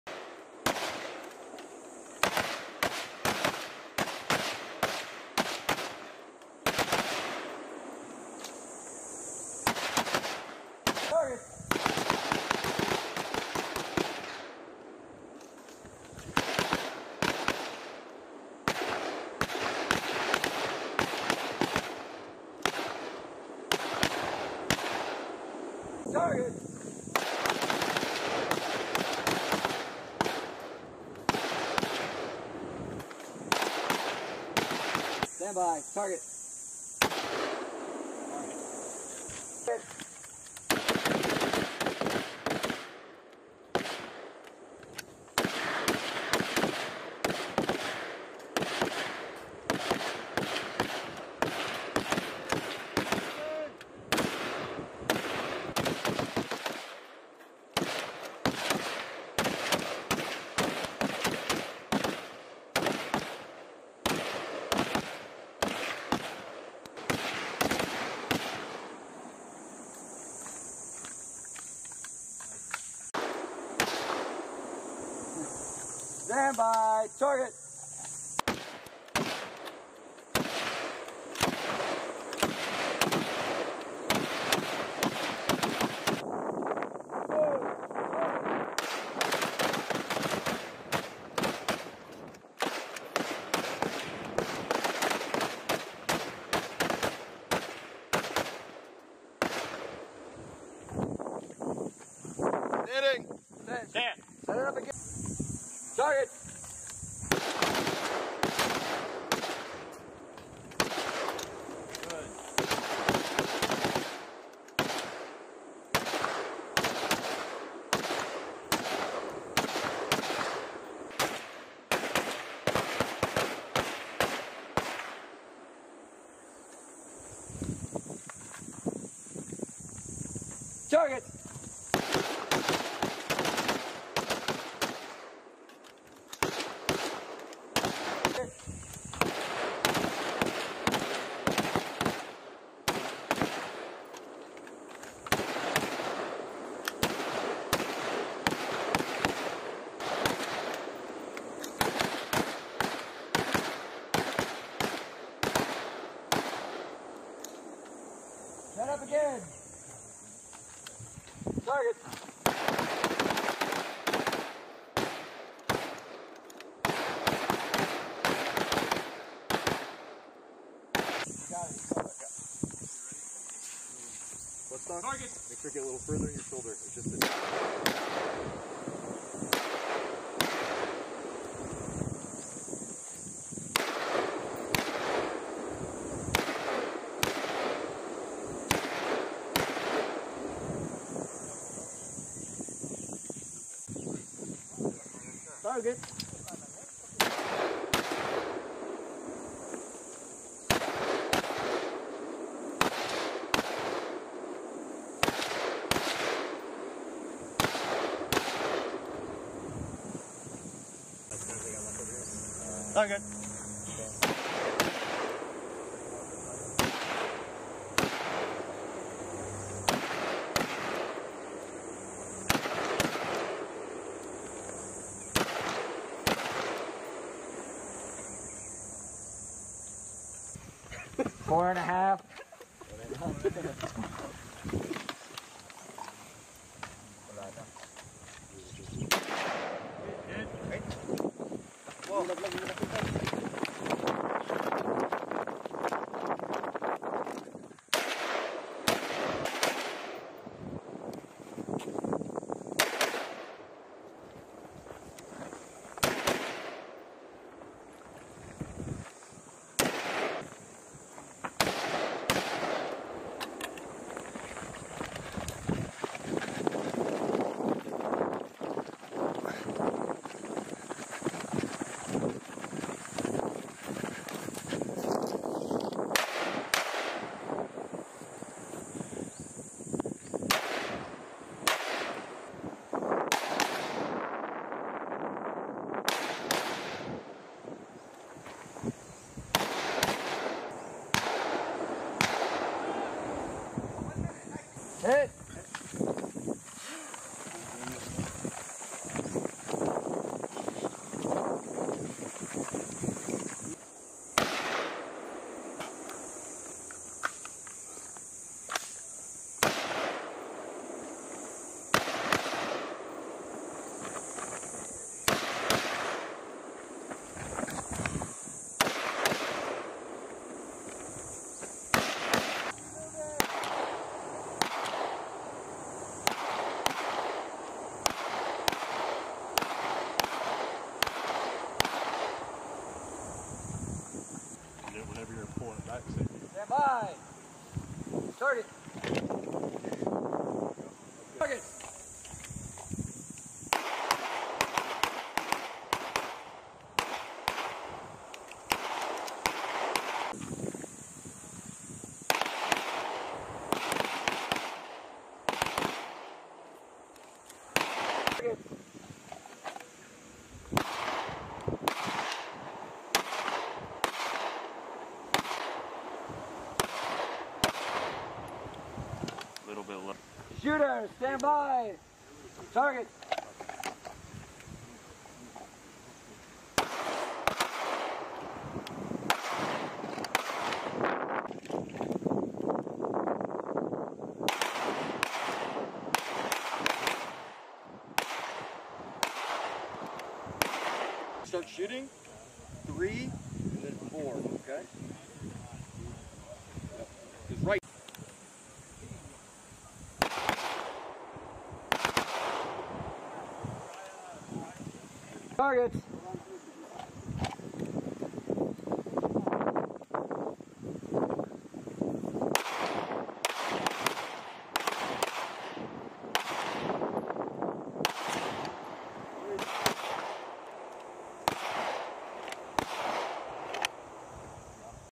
Exercise Balikatan 22 | Philippine Marines, 3d Recon Close Quarters Battle Range
U.S. Marines with 3d Reconnaissance Battalion, 3d Marine Division, and Philippine Marines with Force Reconnaissance Group, conduct a close-quarters battle range during Balikatan 22 at Marine Barracks Gregorio Lim, Cavite, Philippines, April 2, 2022.

Balikatan 22: Philippine Marines, 3d Recon Close Quarters Battle Range
PHILIPPINES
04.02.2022

 Balikatan is an annual exercise between the Armed Forces of the Philippines and U.S. military designed to strengthen bilateral interoperability, capabilities, trust, and cooperation built over decades of shared experiences. Balikatan, Tagalog for ‘shoulder-to-shoulder,’ is a long-standing bilateral exercise between the Philippines and the United States highlighting the deep-rooted partnership between both countries. BK22 is the 37th iteration of the exercise and coincides with the 75th anniversary of U.S.-Philippine security cooperation. (U.S. Marine Corps video by Cpl. Jackson Dukes)

Film Credits: Video by Cpl. Jackson Dukes
Exercise Balikatan